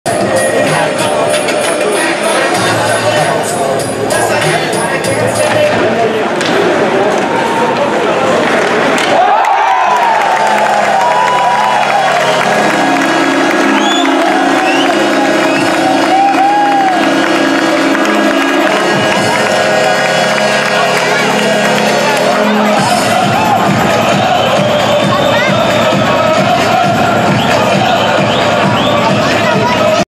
2e But de la saison de Stefan Chaput des Marquis de Jonquiere 08-10-2016 LNAH
Le Blizzard de Trois-Rivieres vs Les Marquis de Jonquiere LNAH
Victoire des Marquis 6-1